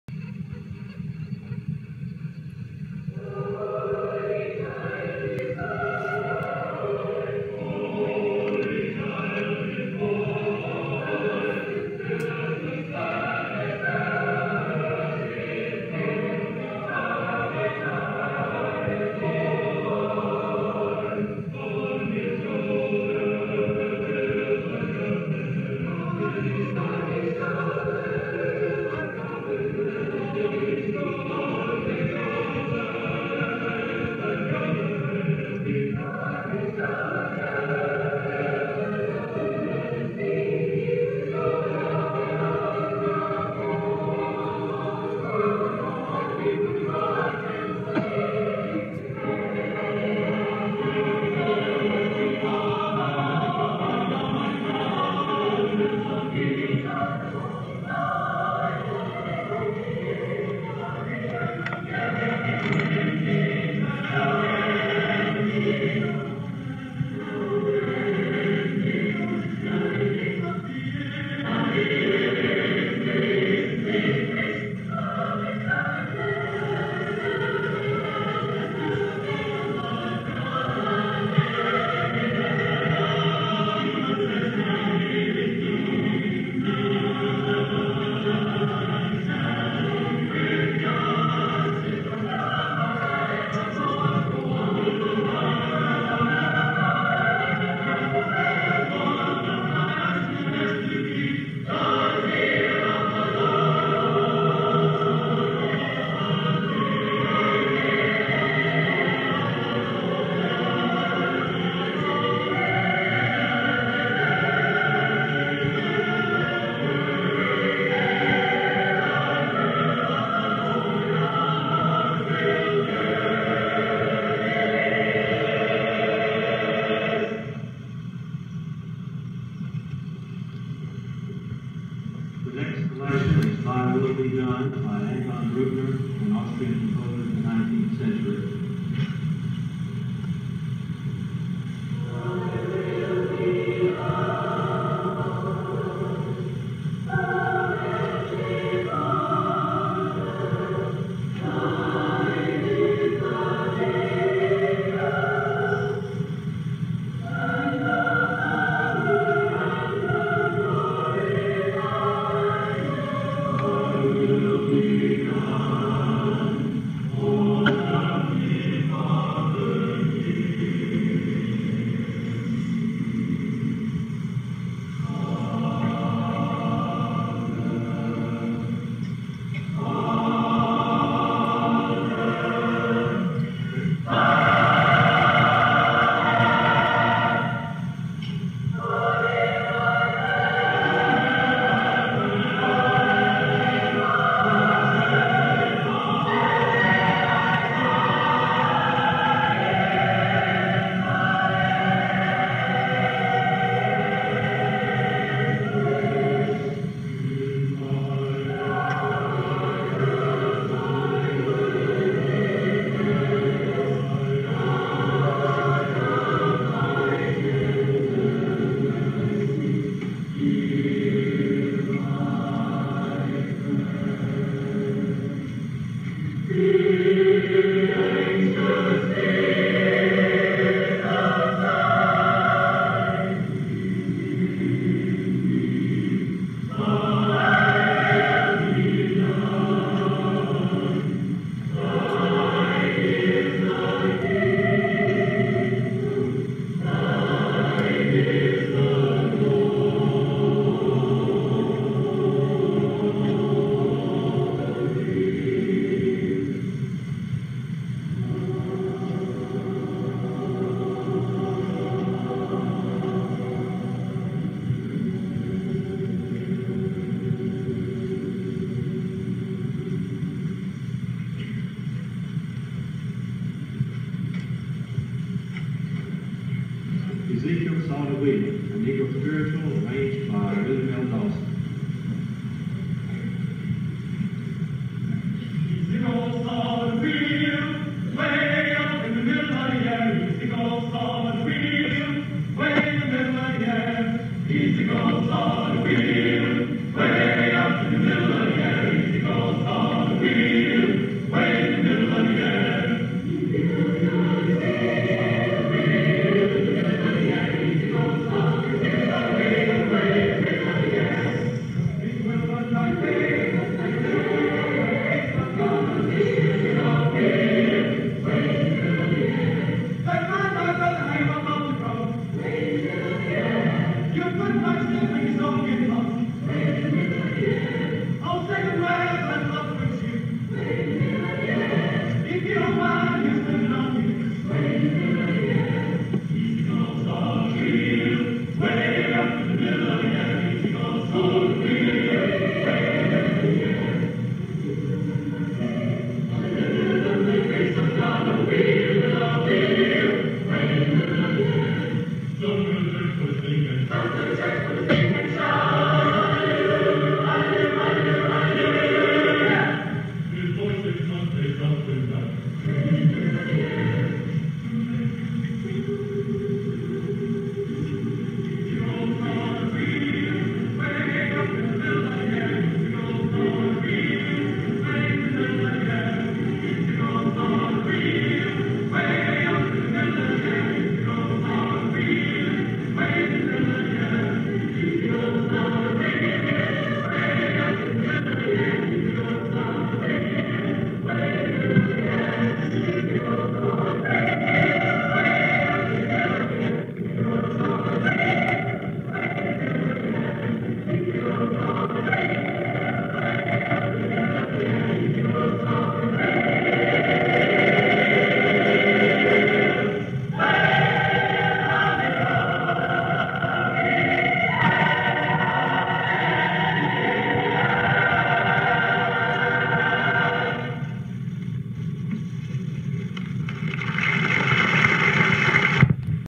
Freed Hardeman University A Capella Chorus 1978
FHU A Capella Chorus under the direction of John Hall during the 1978 lectureship